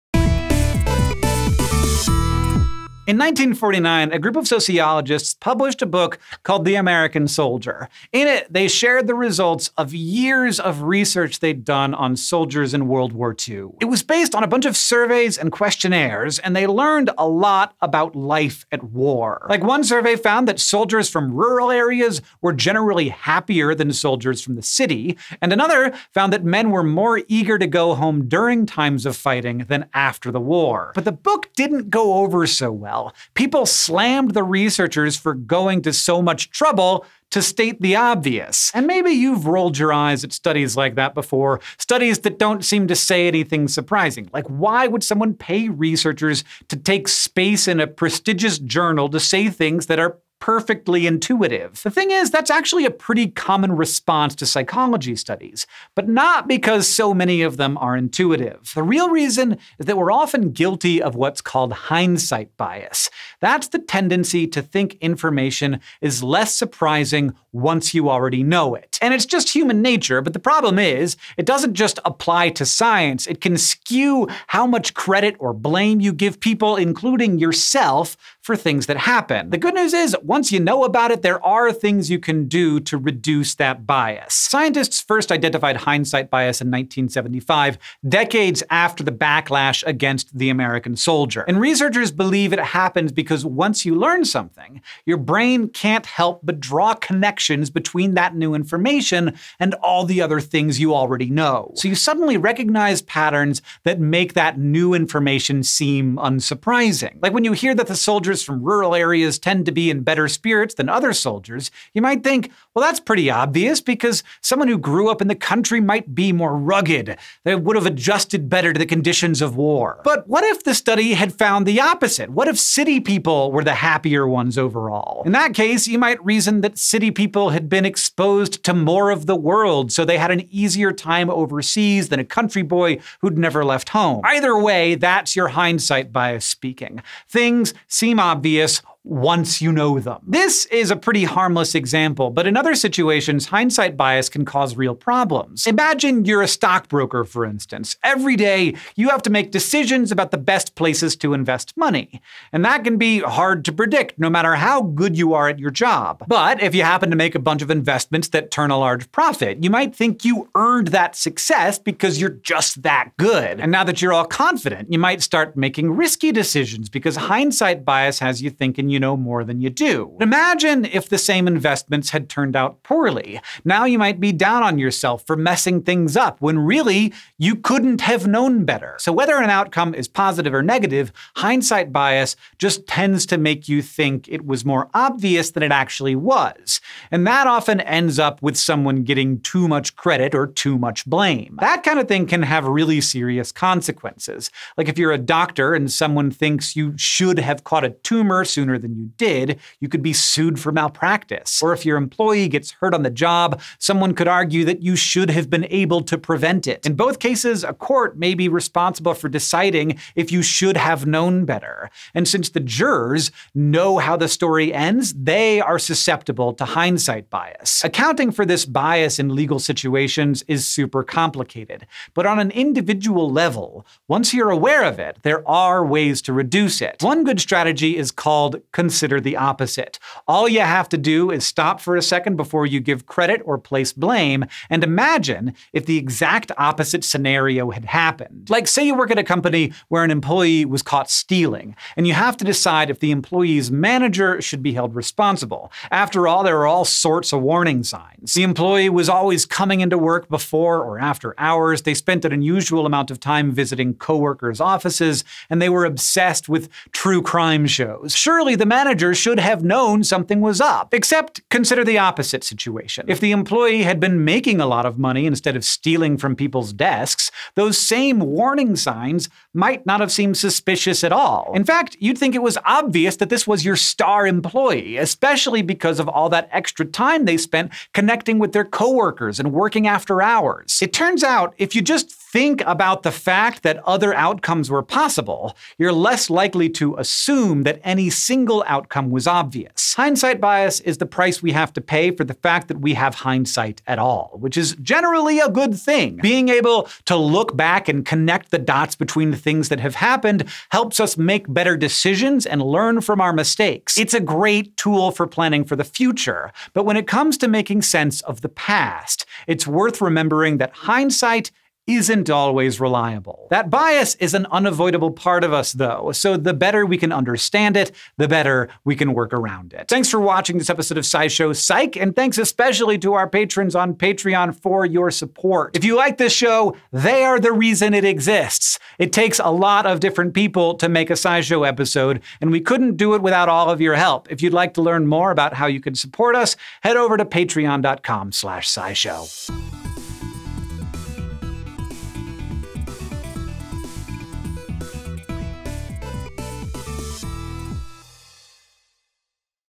Why Psychology Tells Us What We Already Know
Hindsight bias skews our interpretation of events and information, making it seem like they were predictable or just not that surprising. This bias can cause some real problems, but the good news is, once you are aware of it, there are some things you can do to reduce its effects.